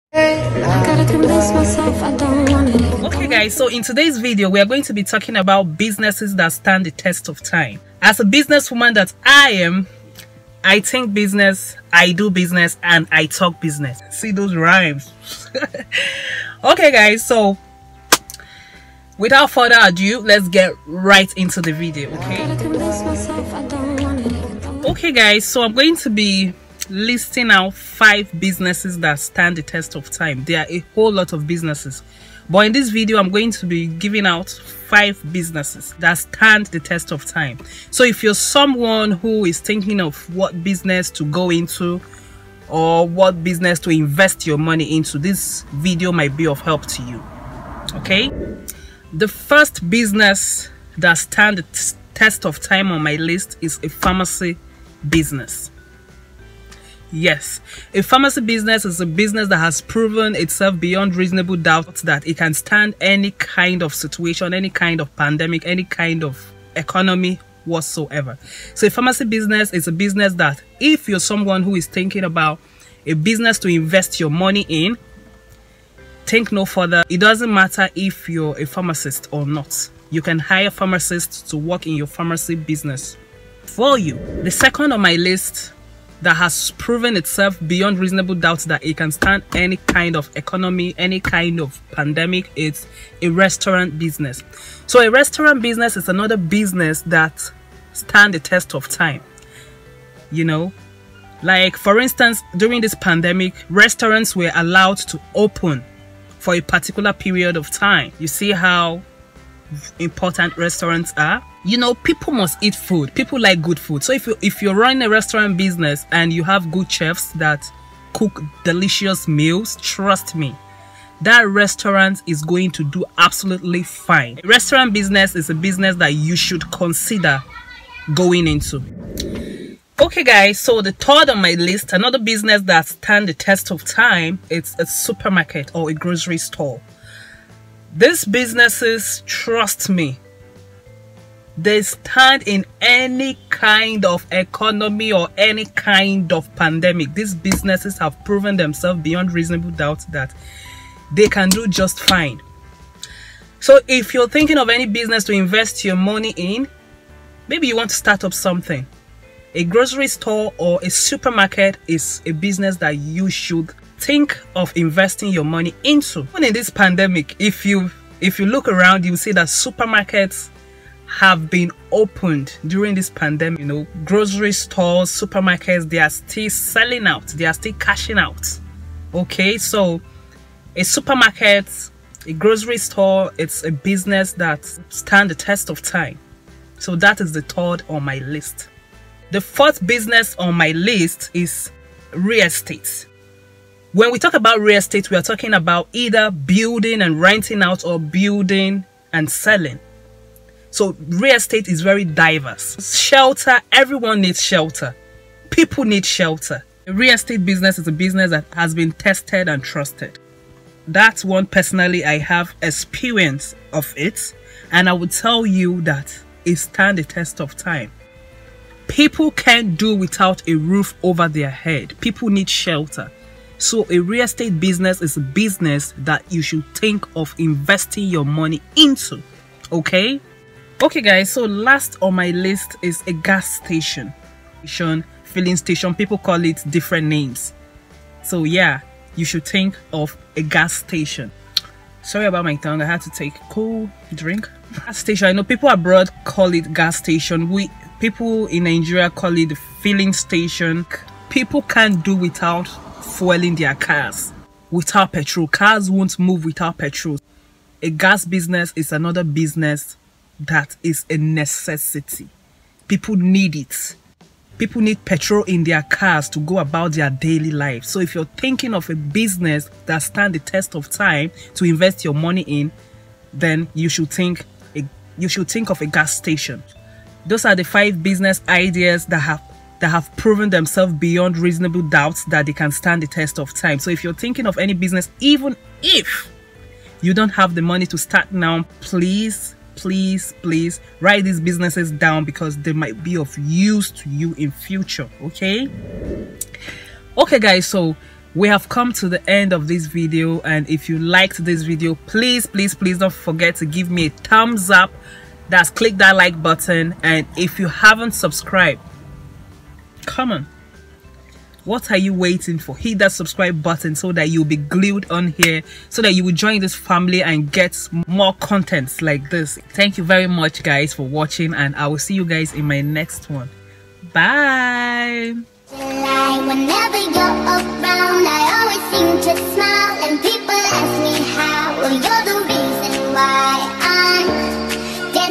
5 Businesses That Stand The Test OF Time.........../Corona ain’t Got Nothing On Them
Hello guy’s! How is everyone doing, so in this video I talked about Businesses that stand the taste of time, I hope you guys enjoy watching.